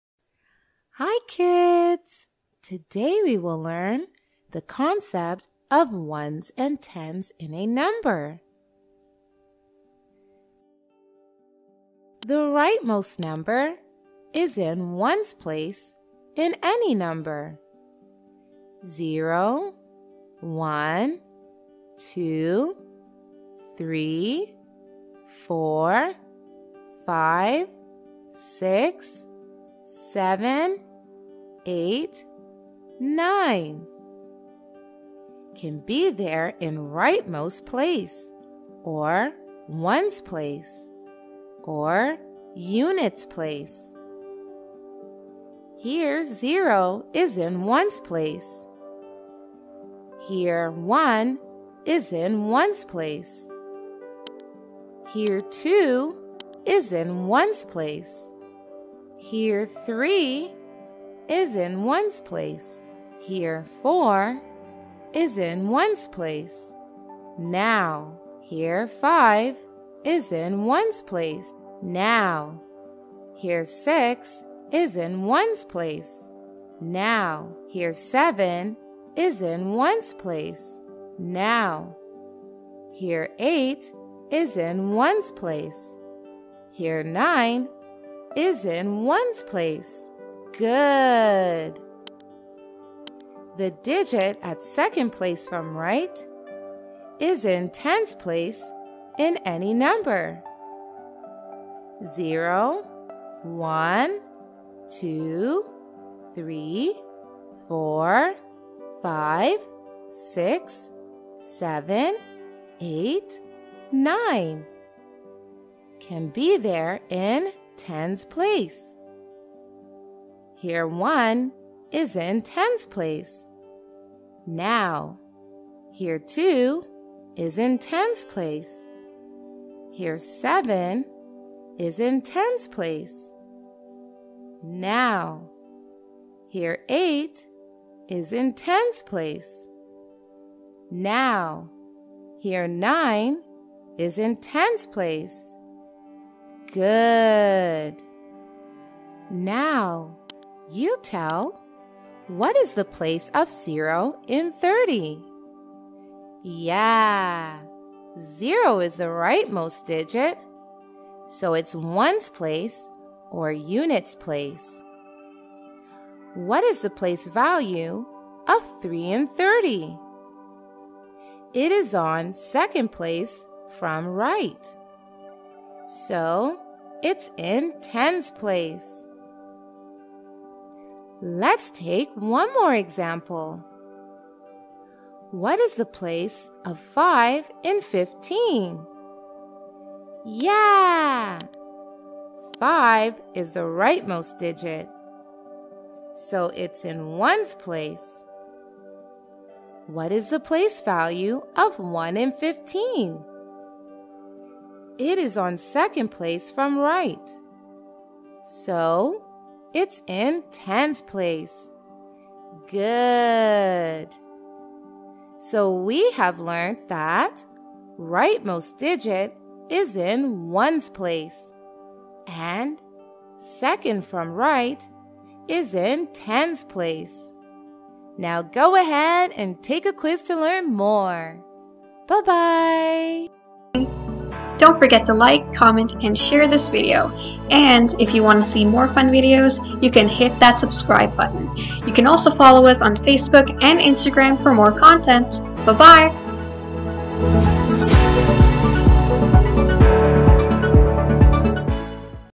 Place Values | Place Values For Kids | Ones, Tens | 2 Digit Numbers
Place Value – Ones and Tens | Math | Grade-1,2
This educational video explains about Place Value. This will help them a lot in their daily routine.

Topics Covered:
Place and Place Value
Ones, Tens